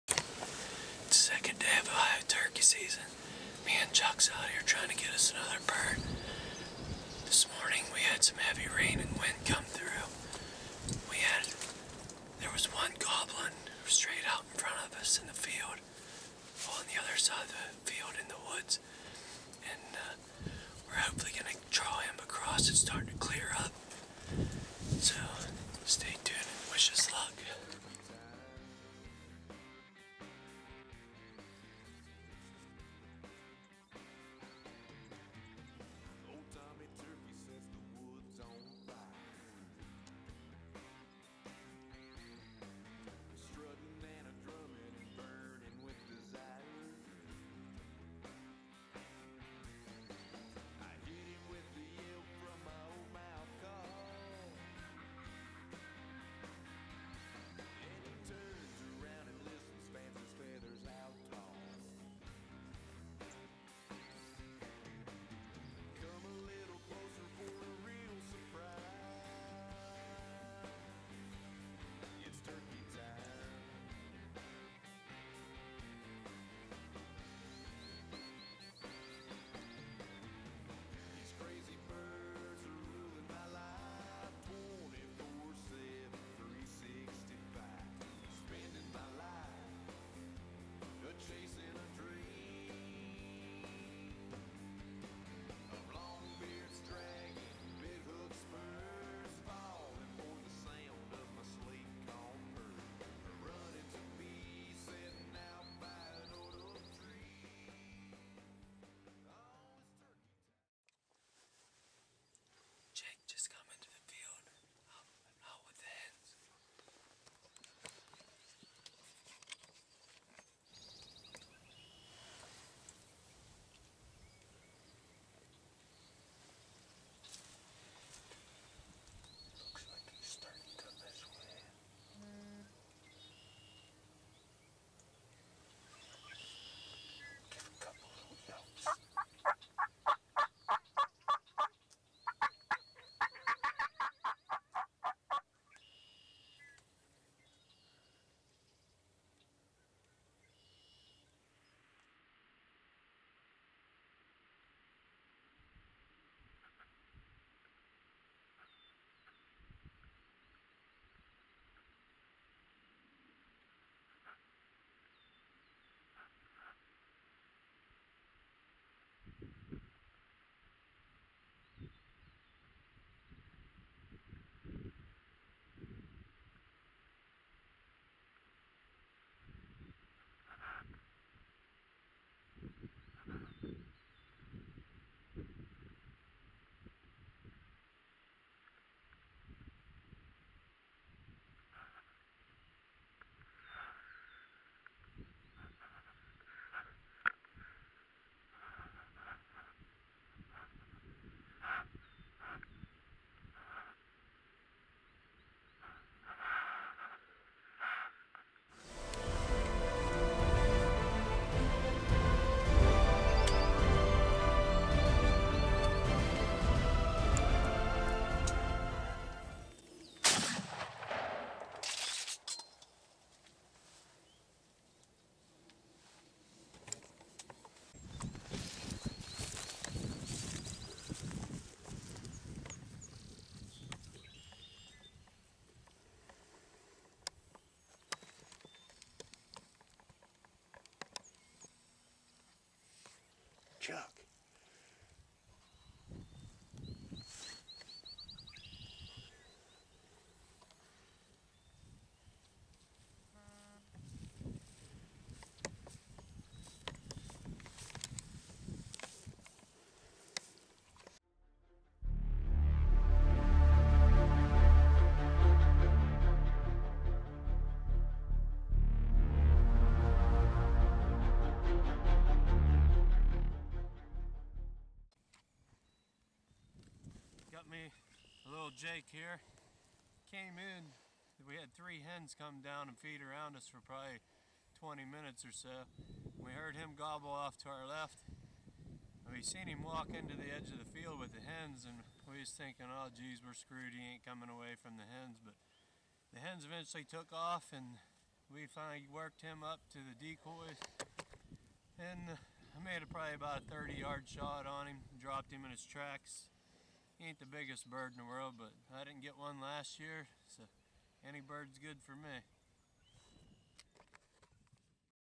OHIO 2015 TURKEY HUNTING
Chuck Board jr. takes a jake during ohio spring turkey season